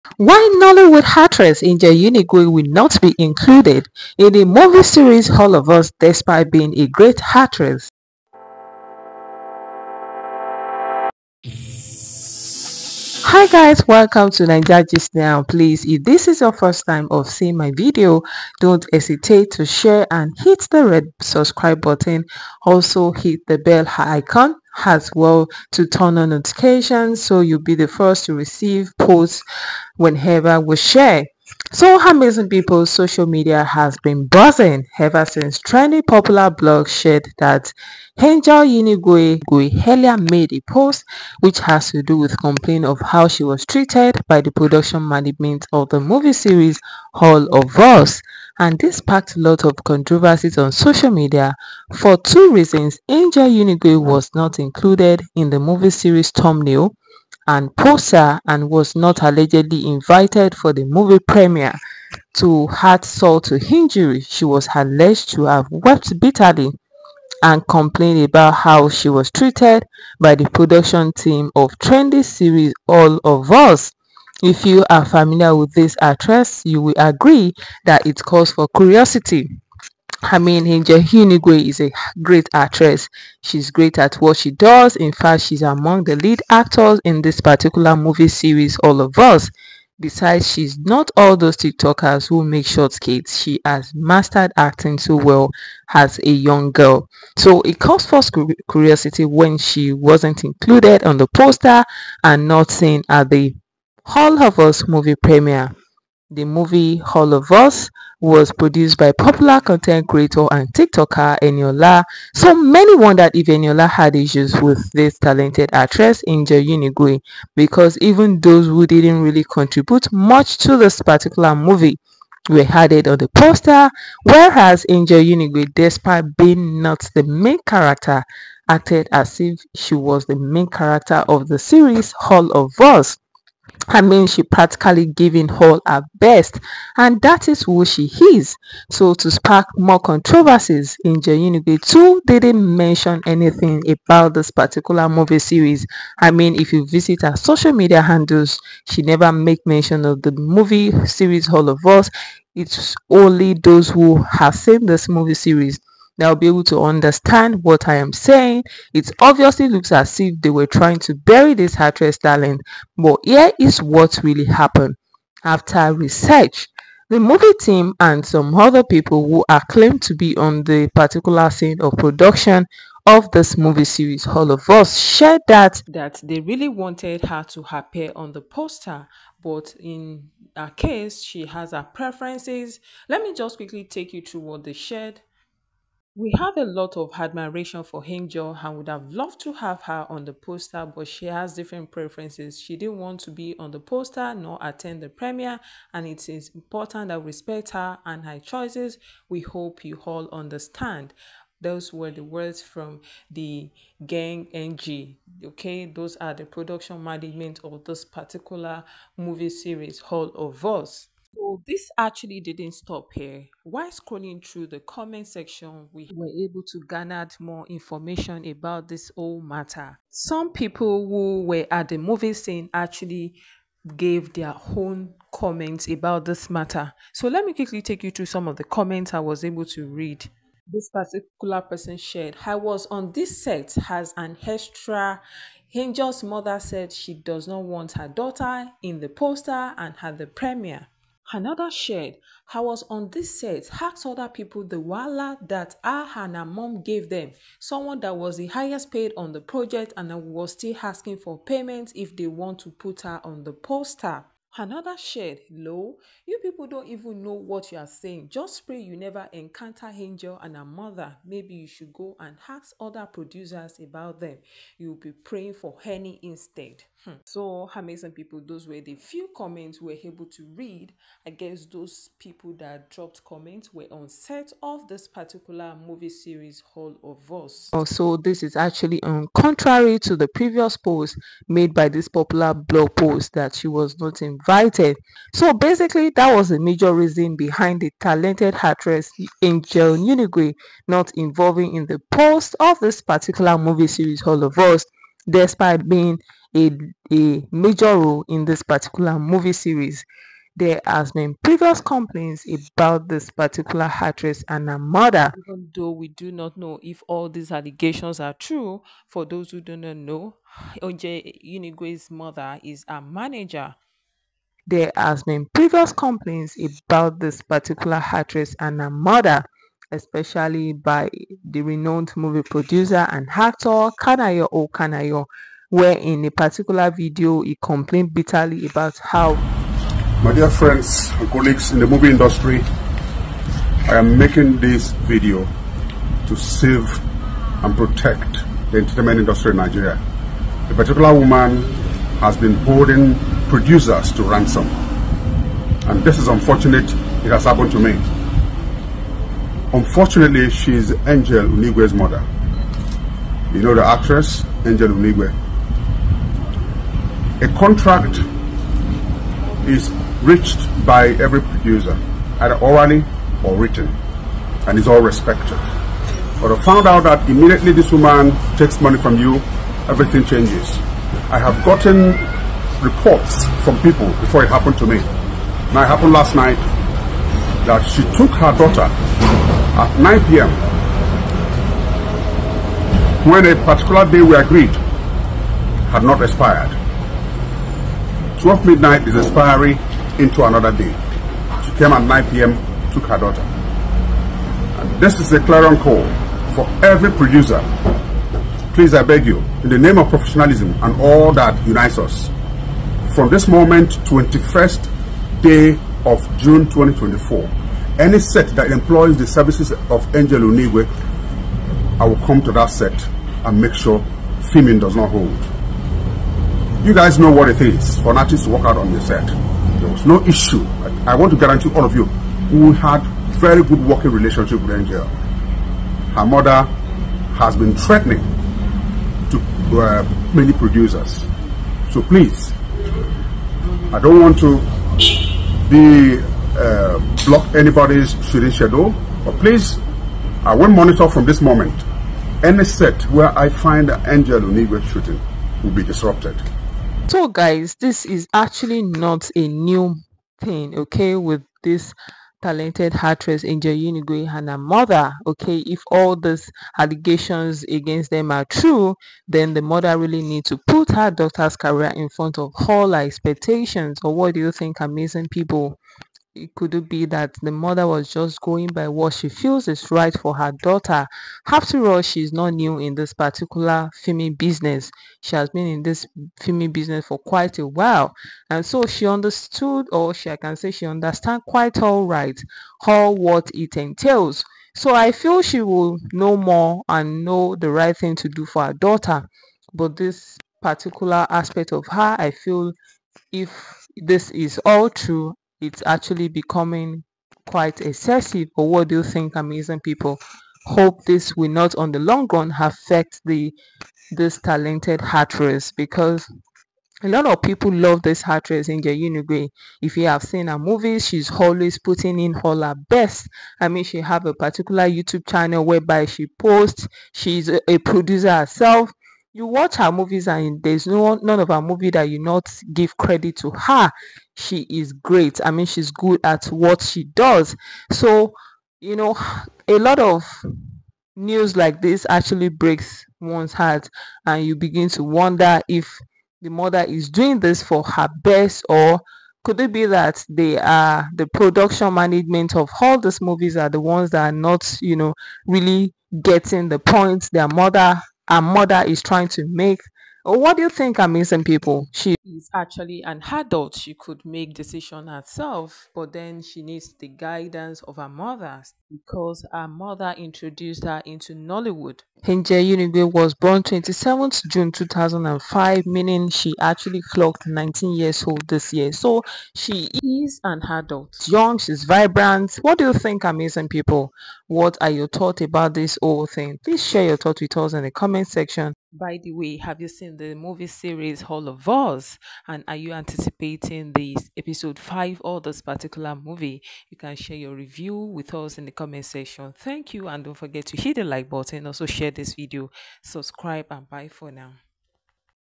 Why Nollywood Actress Angel Unigwe will not be included in the poster of movie series "all of us'"
Social media has been buzzing ever since trendy popular blog shared that angel unigwe earlier complained of how she was treated by the production management of the movie series all of us and this sparked lots of controversies on social media for two reasons, Angel Unigwe was not included in the movie series thumbnail & poster and was not allegedly invited for the movie premiere. To add salt to injury  she was alleged to have wept bitterly and complained about how she was treated by the production team of trendy series all of us ...

All of us latest Nigerian series
All of us Nigeria series full movie